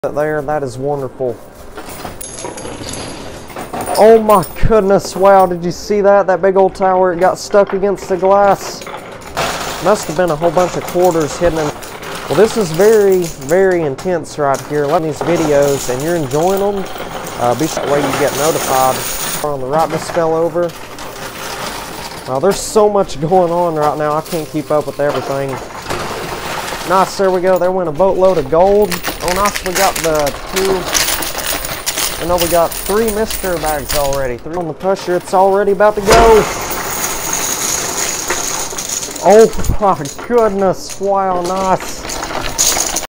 massive tower stuck…
Huge tower of poker chips got stuck we won millions at the coin pusher machine!